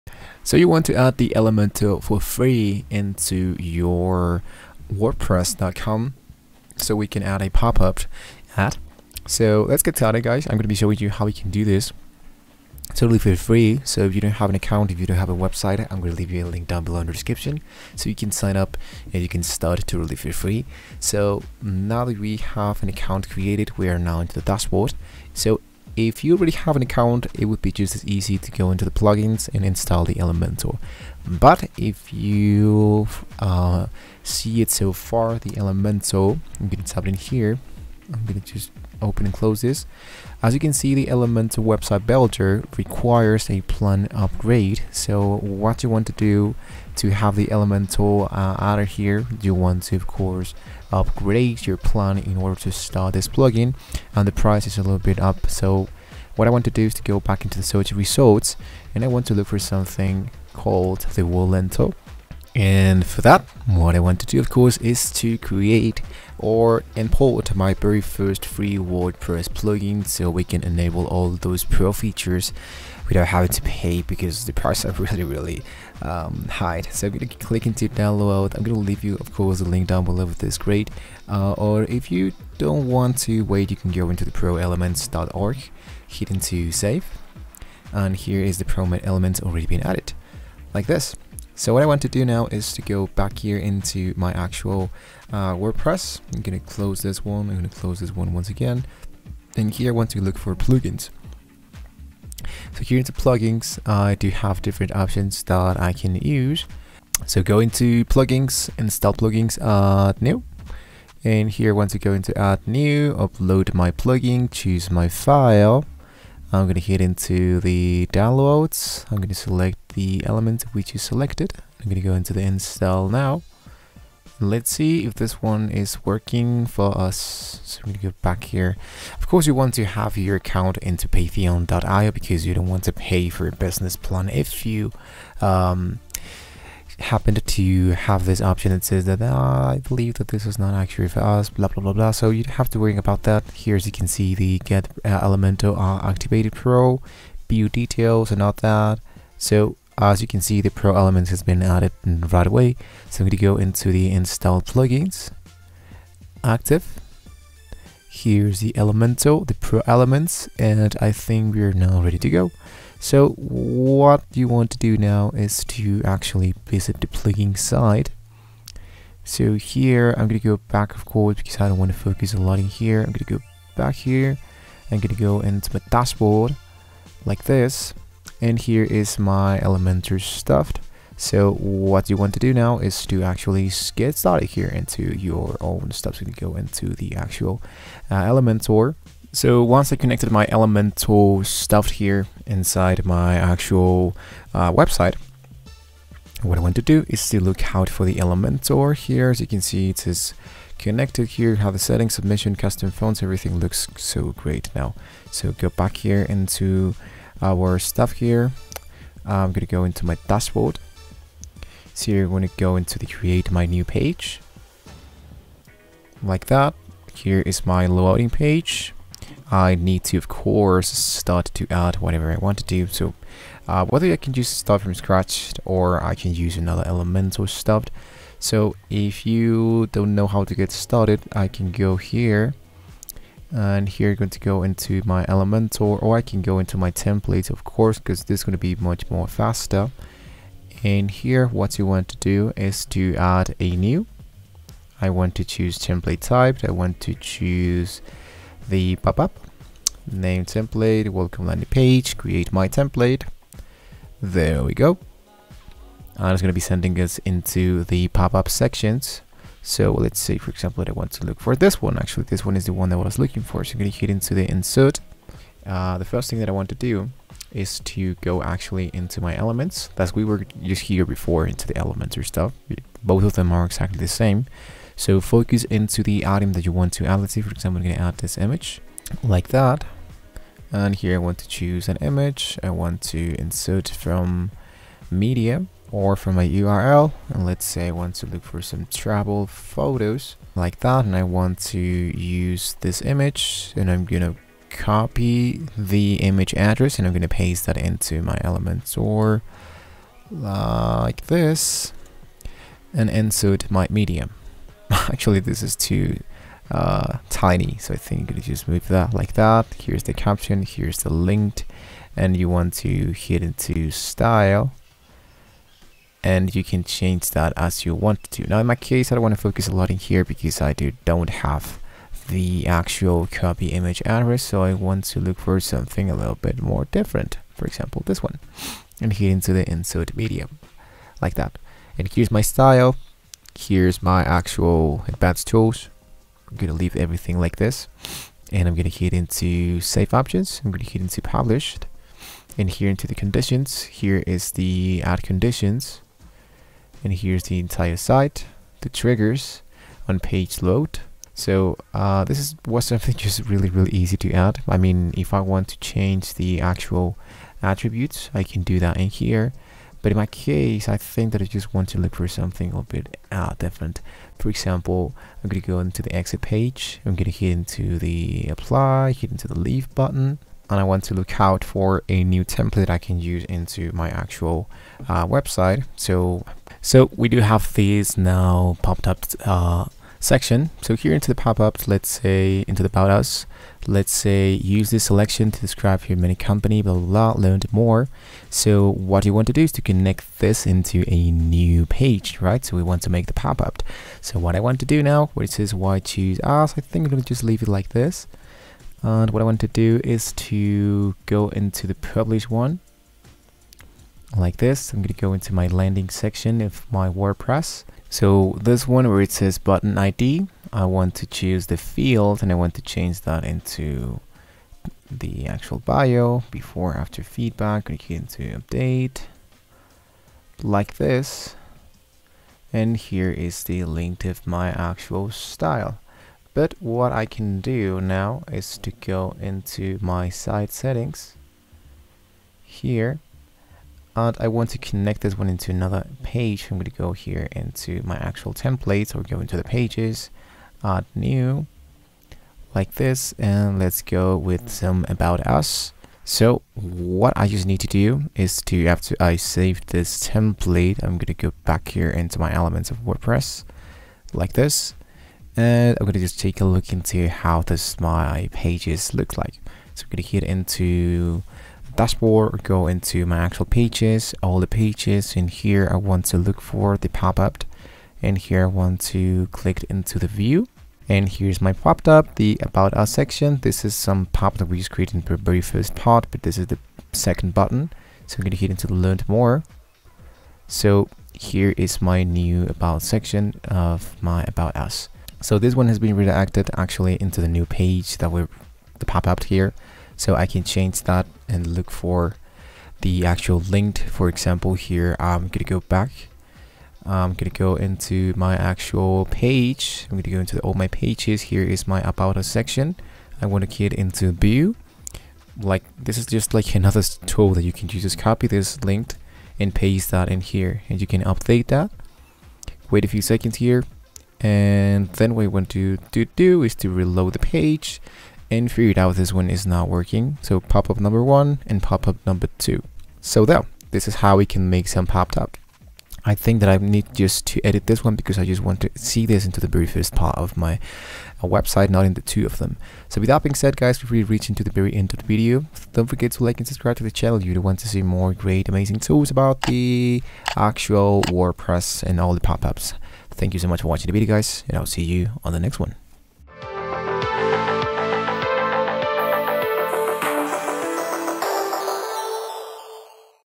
How To Create A Popup In Elementor (2024) Tutorial For Beginners
Creating a popup in Elementor (2023) is a quick and easy process that can be accomplished in just a few simple steps. First, open the Elementor dashboard, and select the “Popup” option. This will bring up a new popup editor window, allowing you to customize the content of your popup. You can add text, images, videos, and more.

Once you’re happy with the design of your popup, select the “Publish” option to make it live. You can also set a time frame or conditions for when or how your popup will appear. After you have successfully created your popup, you can test it by visiting your website. With these simple steps, you can create a professional-looking popup in Elementor (2023) with ease. ‎